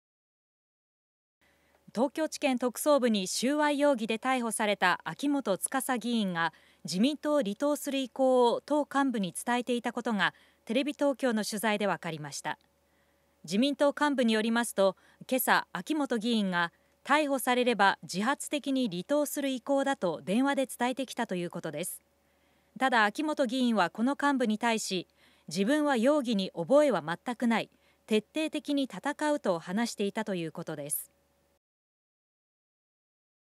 【独自】秋元司 衆院議員が自民党離党の意向伝える
東京地検特捜部に収賄容疑で逮捕された秋元司衆院議員が、自民党を離党する意向を党幹部に伝えていたことがテレビ東京の取材で分かりました。
自民党幹部によりますと、けさ、秋元議員が、「逮捕されれば、自発的に離党する意向だ」と電話で伝えてきたということです。
ただ、秋元議員は、この幹部に対し、「自分は容疑に覚えは全くない。徹底的に戦う」と話していたということです。